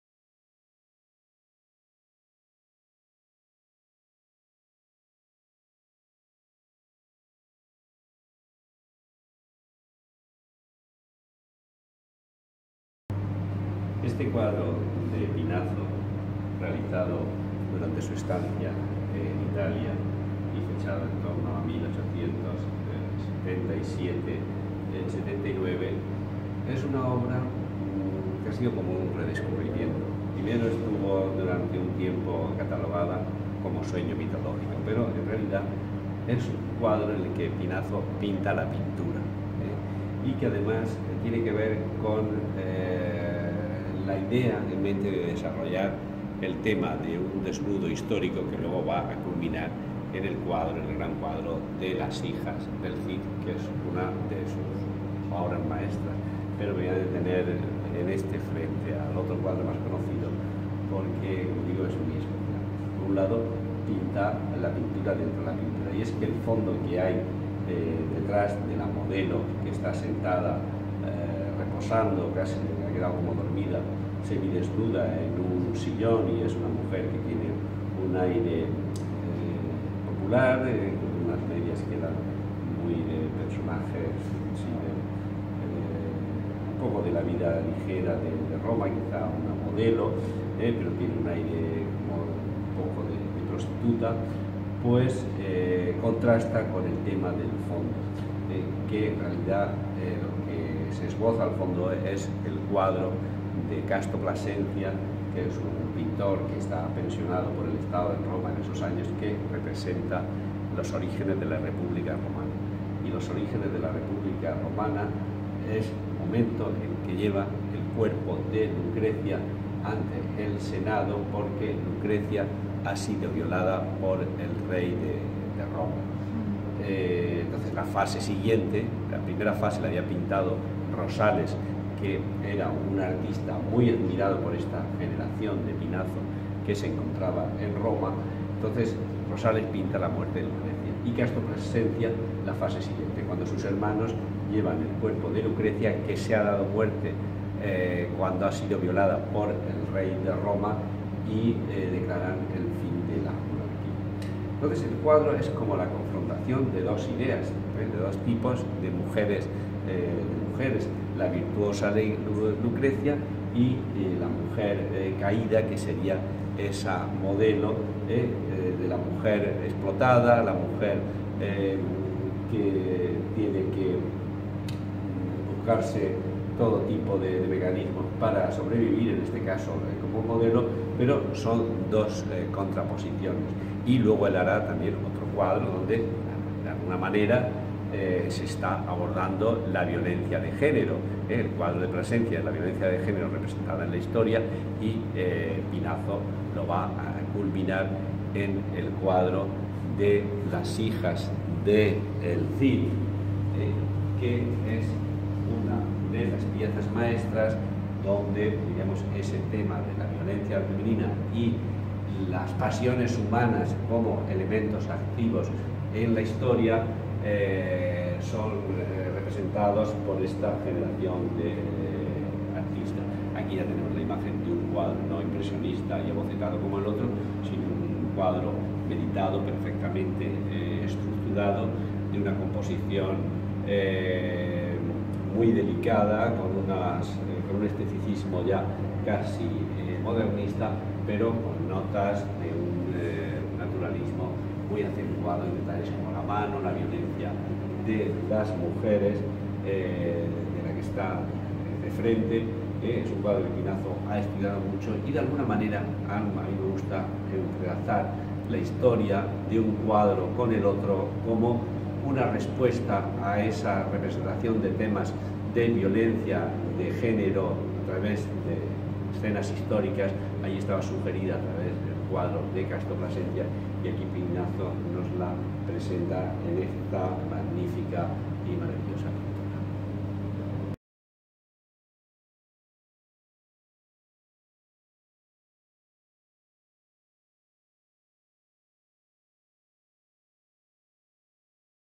EL DESNUDO EN LA OBRA DE IGNACIO PINAZO. Unas palabras de...
El comisario de la exposición temporal, Javier Pérez Rojas, comenta dos de las obras más emblemáticas de la muestra.